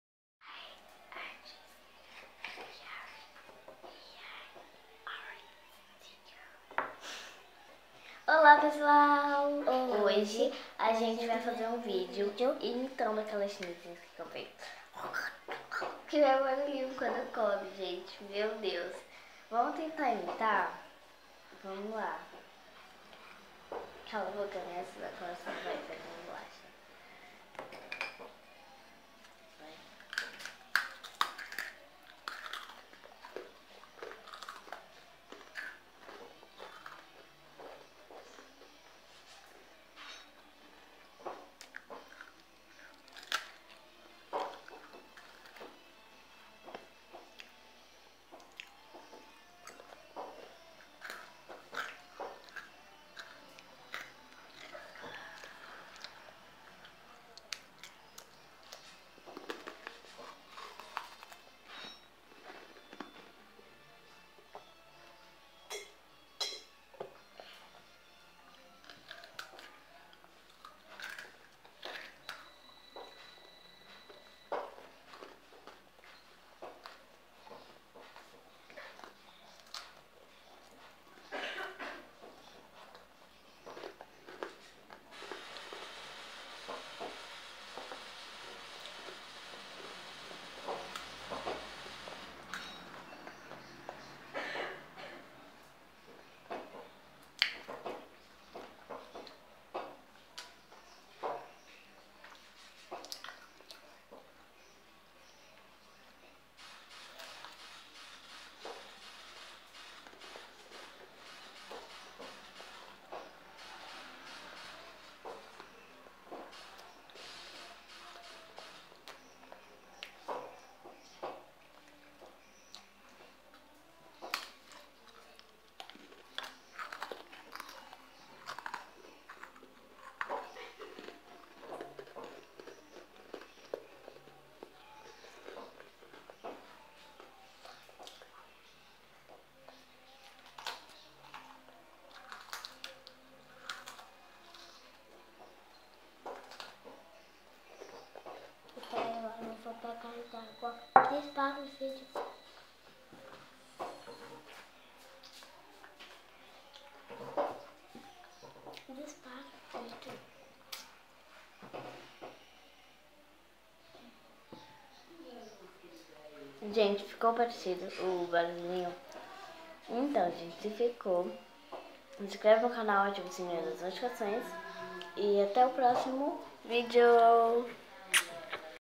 Imitando asiáticas comendo fazendo barulho Olha como ficou engraçado
Oi gente é para você ter uma noção Zinha que Esse vídeo foi não é patrocinado por nenhuma loja nada não ok